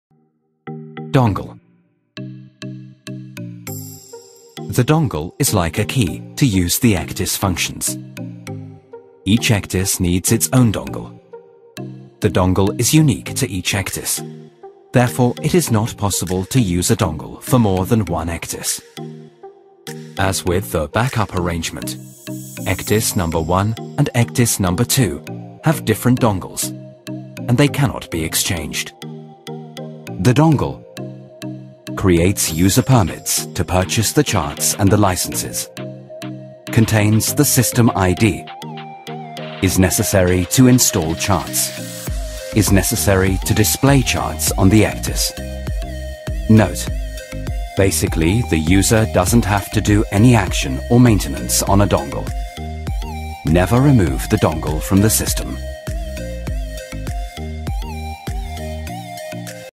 ECDIS Training and Familiarization / Tokyo Keiki EC-8100/8600 / (1.2.3 DONGLE)
1.2.3 DONGLE
-The Dongle is like a key to use the ECDIS functions. Each ECDIS needs its own dongle.
-The dongle is unique to each ECDIS, therefore it is not possible to use a dongle for more than one ECDIS.